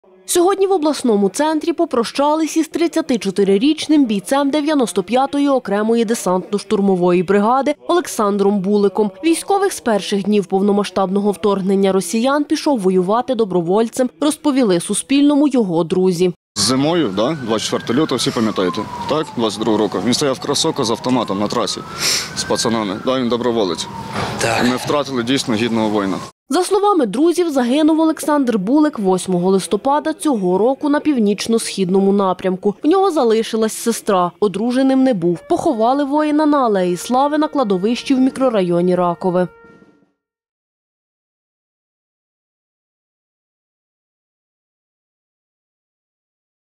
З військовим Олександром Буликом попрощались у Хмельницькому
В обласному центрі попрощались із 34-річним бійцем 95-ї окремої десантно-штурмової бригади Олександром Буликом. Військовий з перших днів повномасштабного вторгнення росіян пішов воювати добровольцем, розповіли Суспільному його друзі.